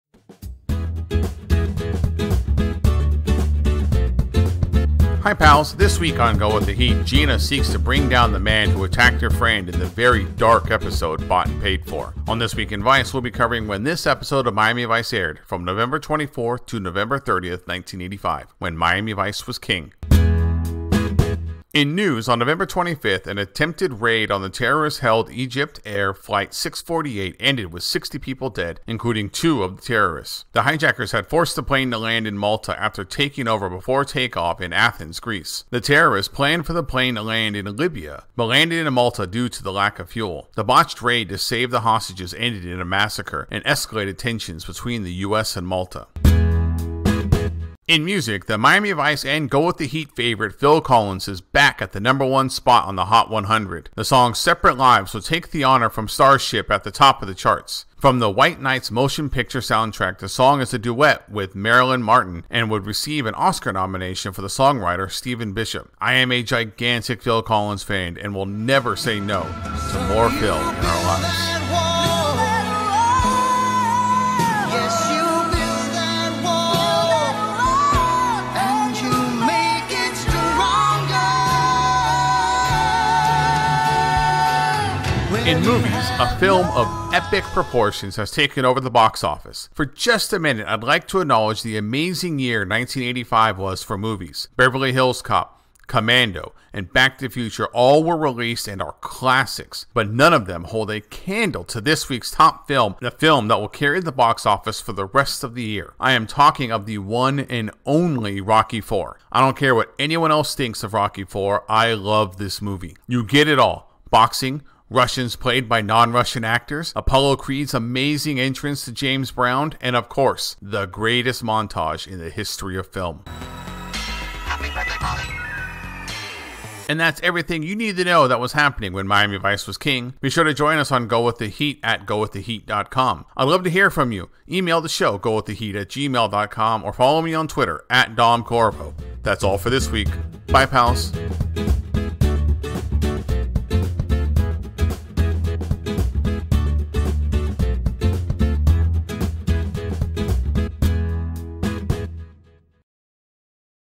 This Week In Vice 20 - Nov. 24-30, 1985
Hi pals, this week on Go With The Heat Gina seeks to bring down the man who attacked her friend in the very dark episode Bought And Paid For. On This Week in Vice we'll be covering when this episode of Miami Vice aired, from Nov. 24 to Nov. 30, 1985, when Miami Vice was King.

Insta and Snap - domcorriveau